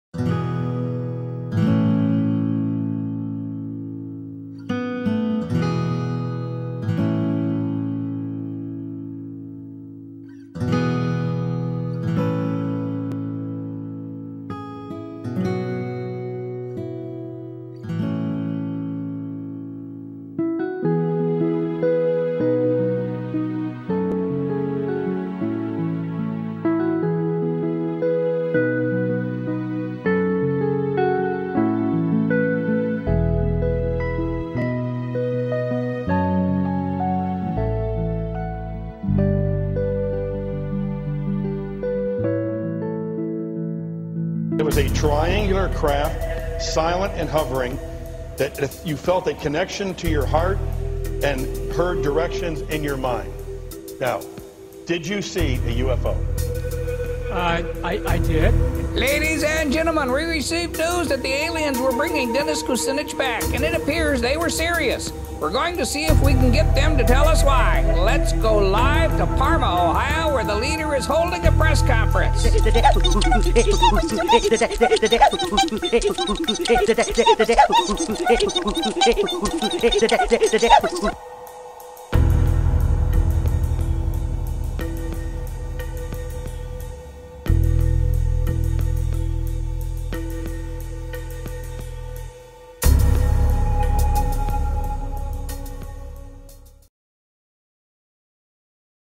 Where's Dennis?.mov
Where has Dennis Kucinich been as Cleveland continues to deteriorate into oblivion? It's time to dump Dennis from Congress!